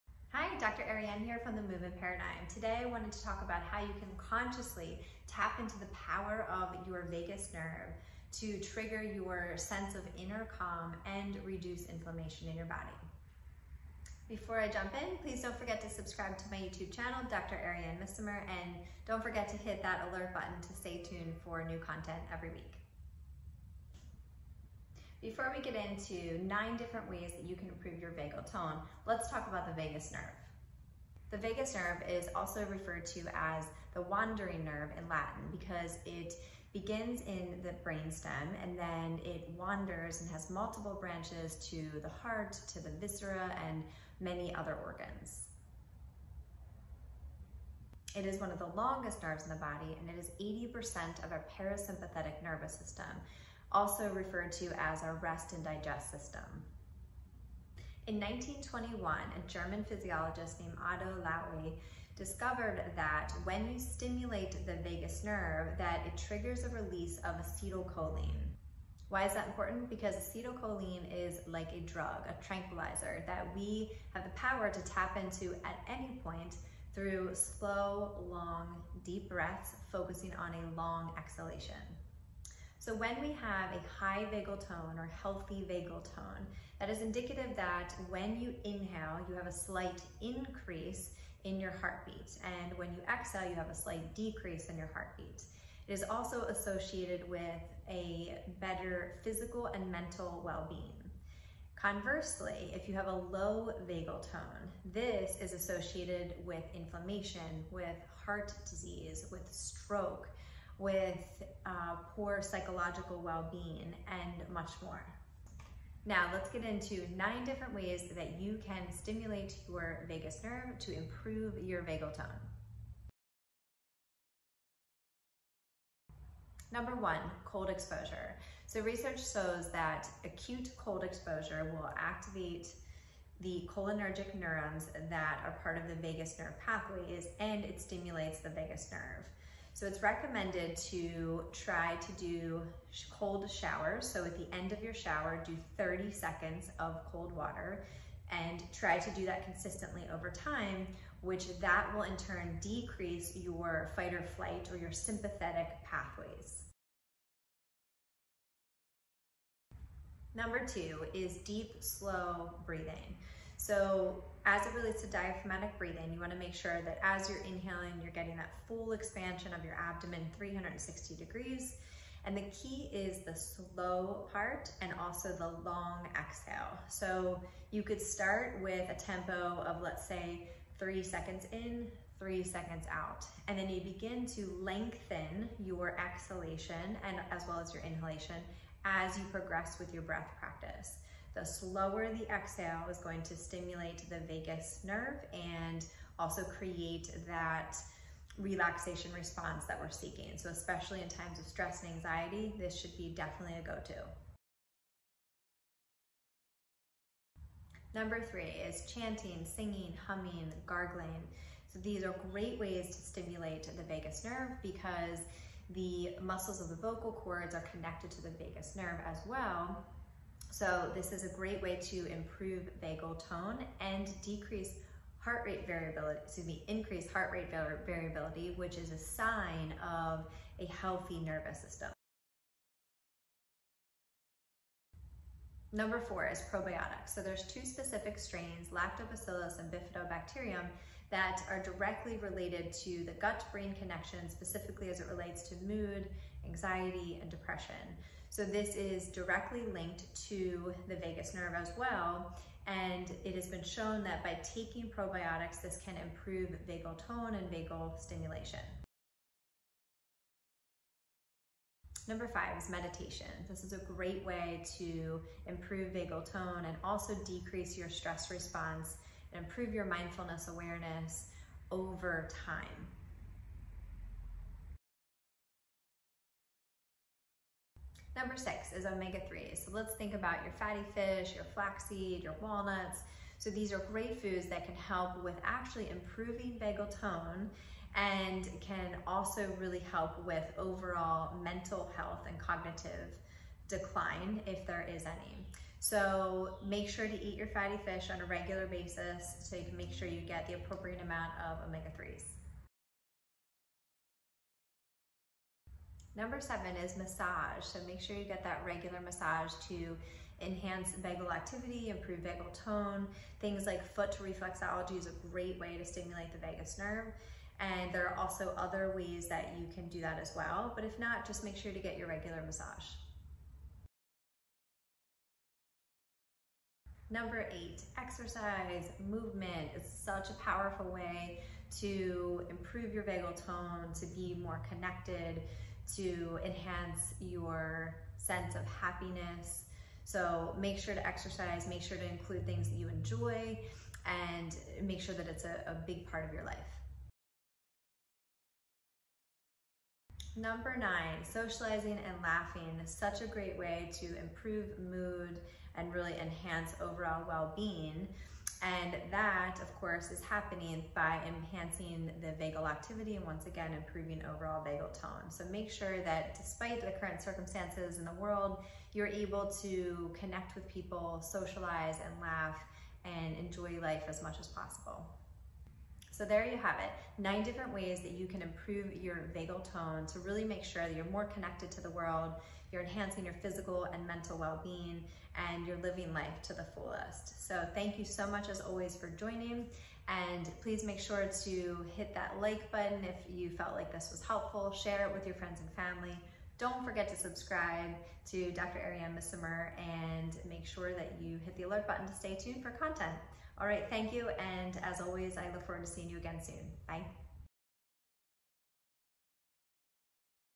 HOW TO IMPROVE YOUR VAGAL TONE | 9 ways to tap into the power of your inner calm
Dr. Arianne discusses various ways from cold exposure to probiotics that you can stimulate your vagus nerve to enhance physical and mental well-being.